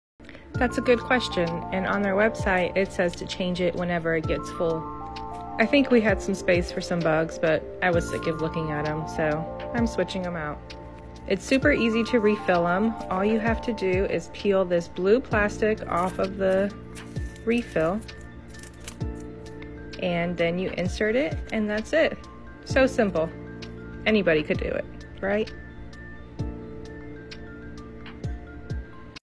Favorite Fly Trap from Zevo Insect
Showing you how easy it is to change the @ZevoInsect Fly Trap.You can find these at Walmart, Target, Home Depot, Lowes, Amazon, etc.It is the only fly trap that I have used that actually works and works quick.